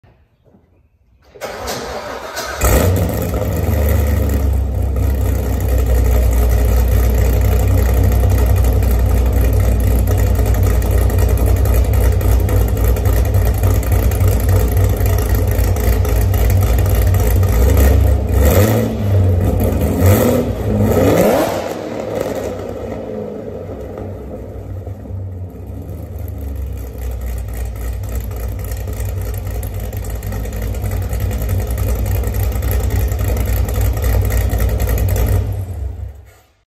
C7 ZO6 Heads Cam Idle
Just installed Soler Performances 103/107 TB. Seems to run perfect still so far after taking the Katech 103 and putting it on my ZL1.

SPICY MEATABALL!